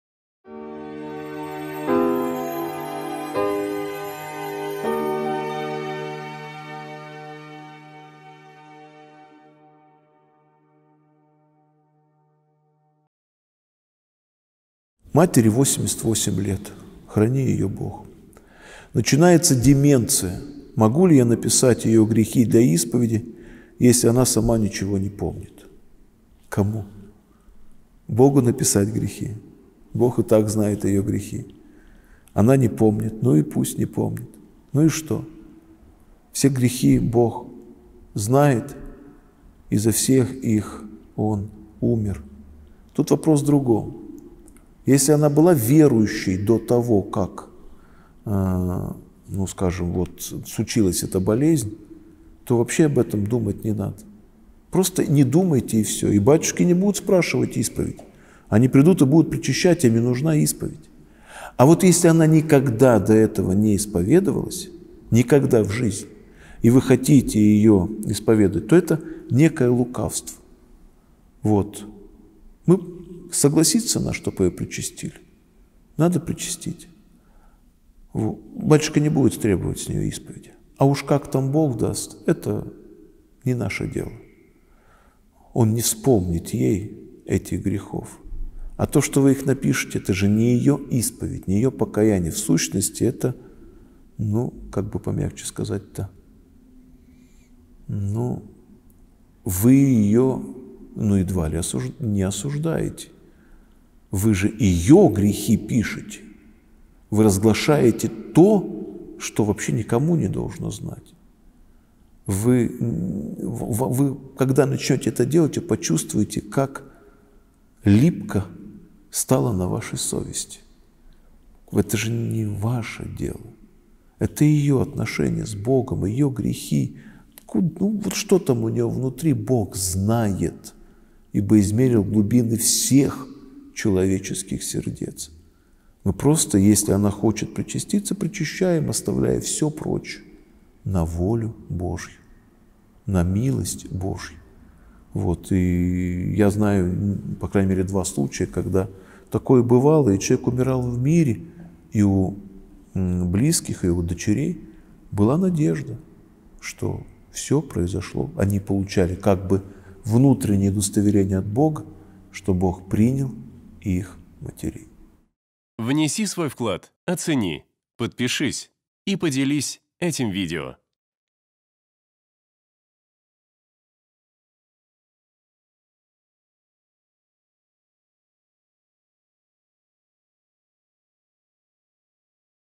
МАМЕ 88 ЛЕТ. ДЕМЕНЦИЯ. МОГУ ЛИ Я НАПИСАТЬ ГРЕХИ ЗА НЕЁ ? Священник Константин Корепанов
Команда Экзегет.

В сегодняшнем видео на портале Экзегет, священник Константин Корепанов
ответит на вопрос: "Маме 88 лет, началась деменция. Могу ли я написать грехи за нее?"

🔸   Бог ведает грехи всех
🔸   Причастие верующих
🔸   Отношения с Господом
🔸   Тайна исповеди
🔸   Воля и милость Божия